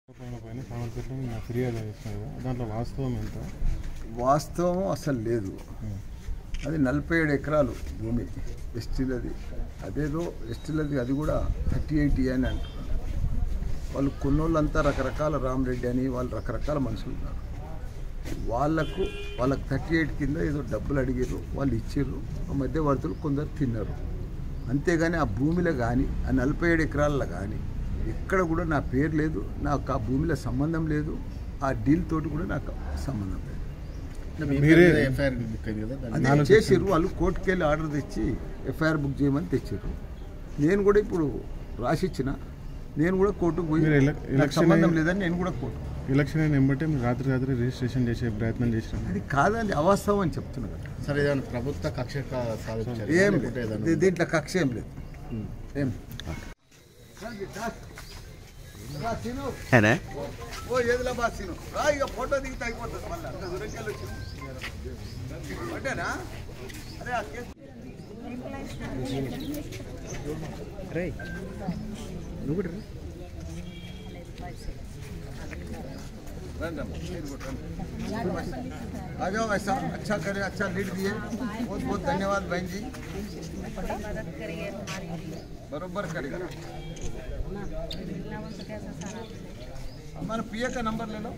నన్ను కాపాడు రేవంత్ రెడ్డి || Malla Reddy Asked Request to CM Revanth Reddy over Land Issue
Stay Updated! 🔔